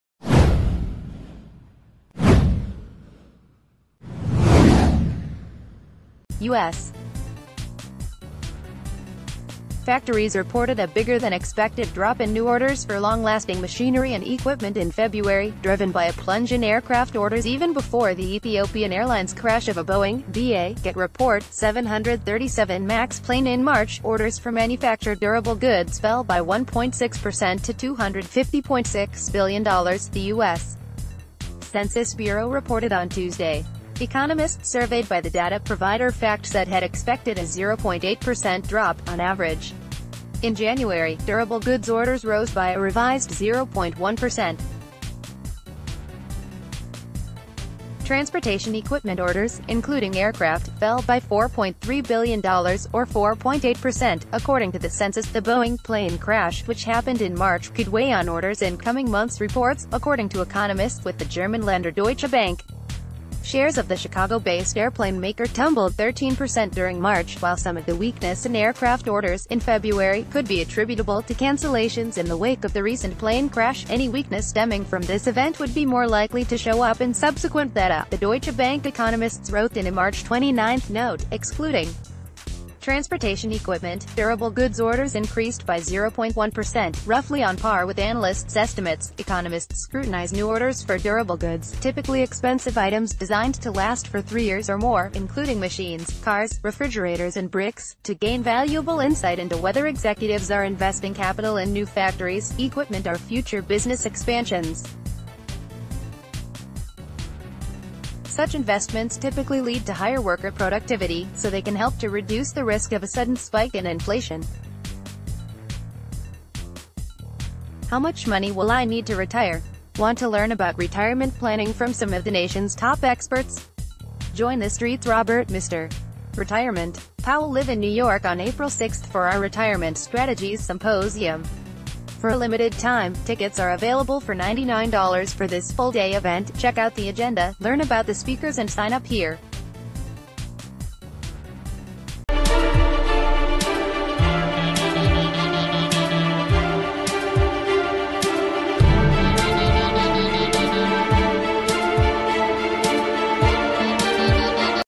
U.S. Aircraft Orders Plunged in February - Even Before Boeing Crash
U.S. factories reported a bigger-than-expected drop in new orders for long-lasting machinery and equipment in February, driven by a plunge in aircraft orders even before the Ethiopian Airlines crash of a Boeing  (BA - Get Report)  737 MAX plane in March.      Orders for manufactured durable goods fell by 1.6% to $250.6 billion, the U.S. Census Bureau reported on Tuesday. Economists surveyed by the data provider FactSet had expected a 0.8% drop, on average. In January, durable goods orders rose b...